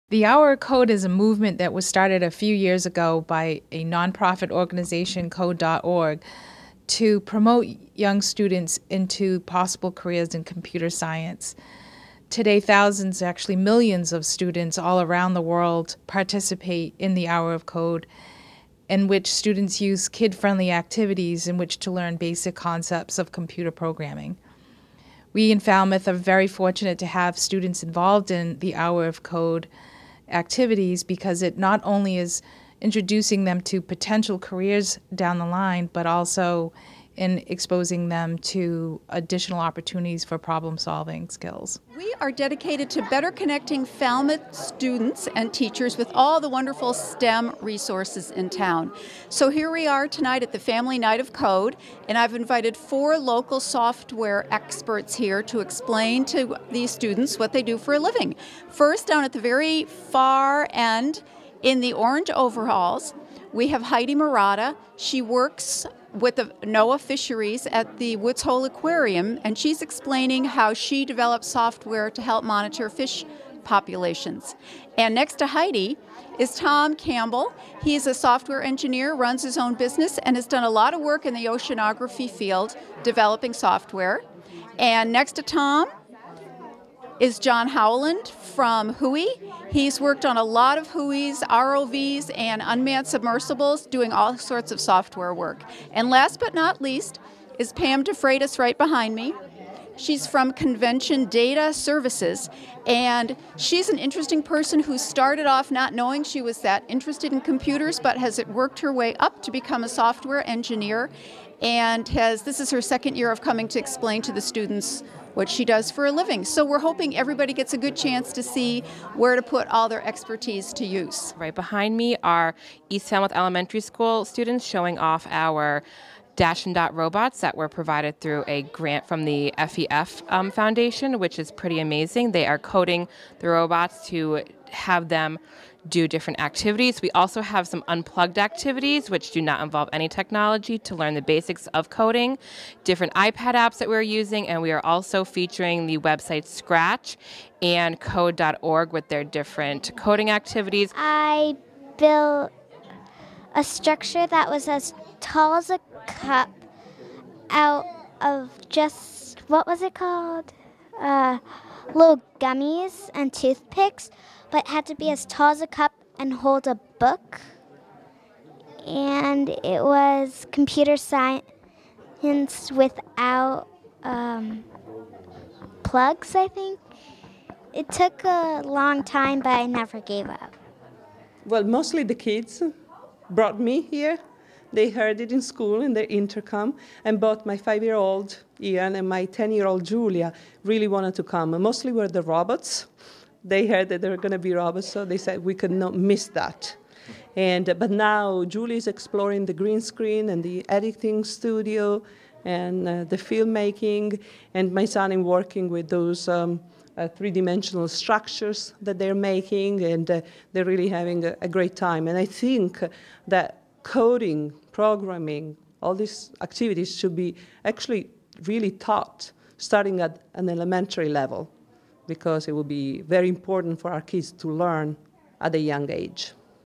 Hour of Code
Falmouth Public Schools families participated in a variety of activities geared toward showing kids programming skills.  In partnership with the Falmouth STEM Boosters, parents and students were able to learn how to code, how it applies in the real world, and speak to local programmers about their jobs.